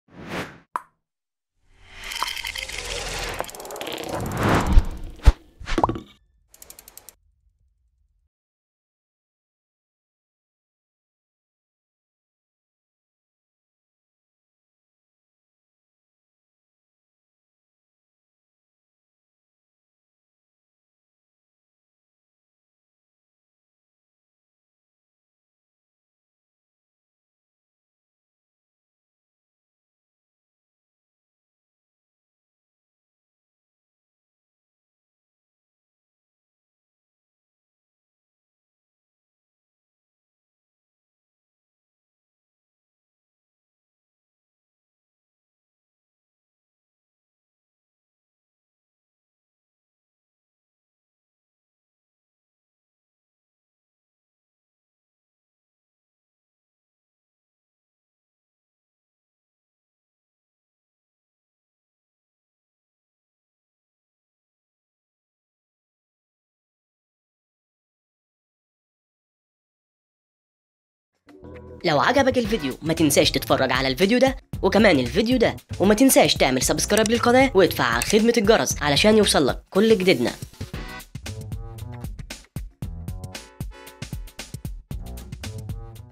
مشاريع افتر افكت مجانية - تحميل قالب بة انترو اكشن رياضي 3d للبرامج الرياضية بشكل مميز 2018
رابط شرح كيفية التحميل من الروابط الخاصة بنا